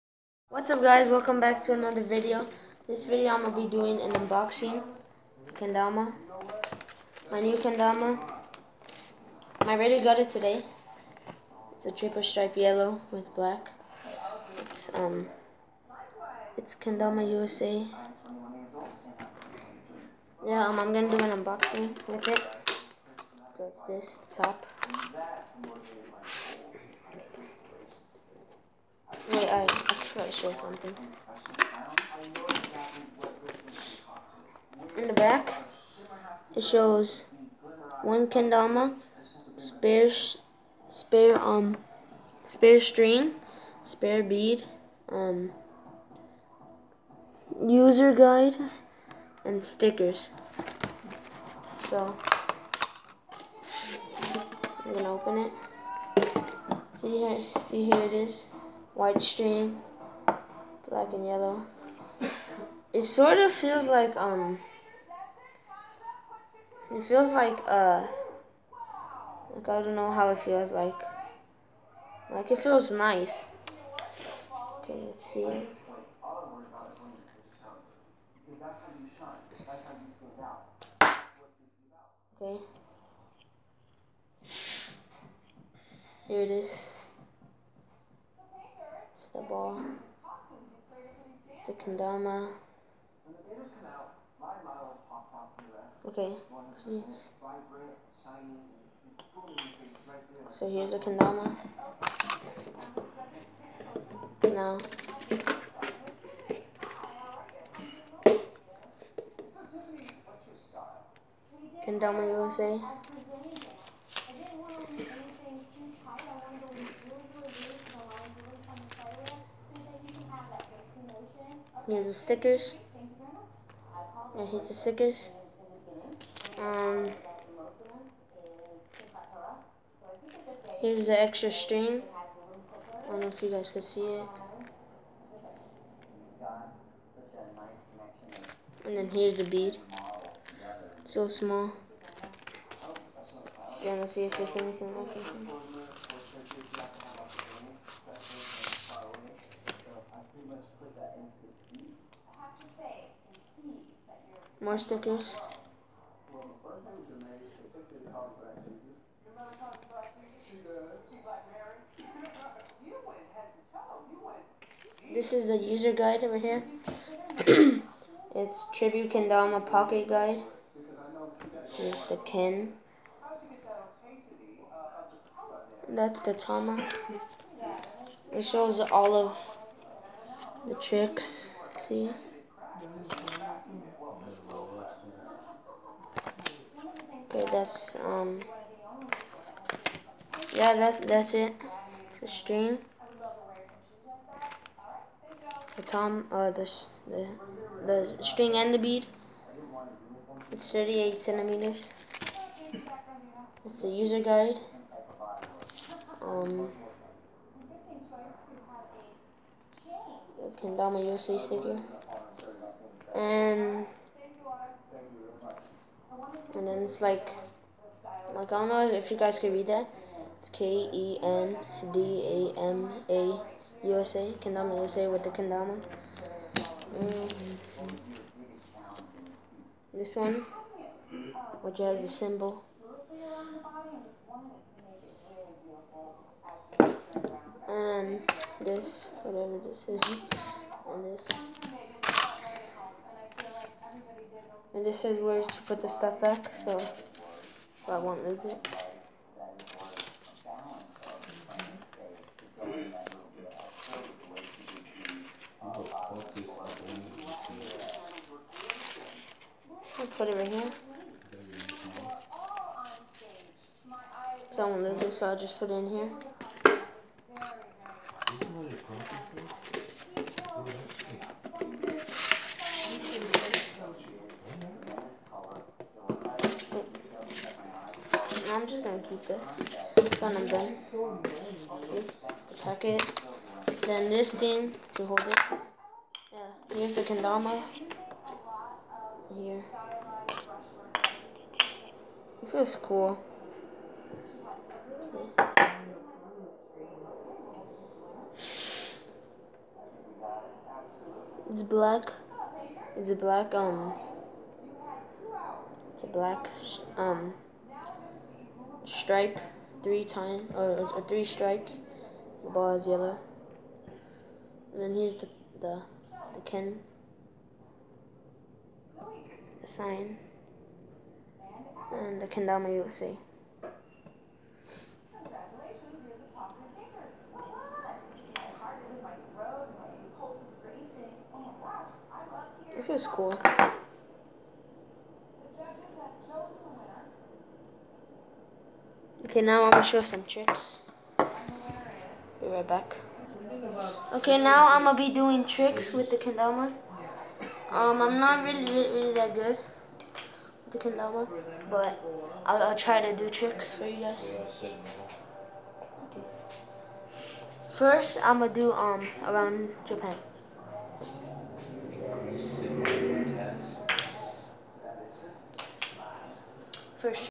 Kendama USA Unboxing
Finally got a triple strip kendama now added new Kendama to my collection make sure to leave a comment down below if you want to see my kendama collection and if you like to see more videos.😄